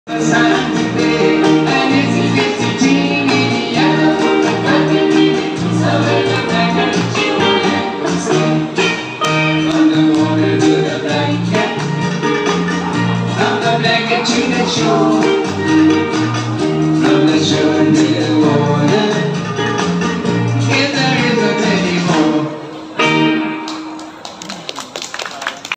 Elvis 2022 & Cliff 2022 at Central Festival Eastville Bangkok Thailand.
อำนาจ ศรีมา กล ภาคสุวรรณ เล็ก เพรสรี่ ดนตรีเปิดหมวกที่ Central Festival Eastville โดยสมาคมนักดนตรีฯ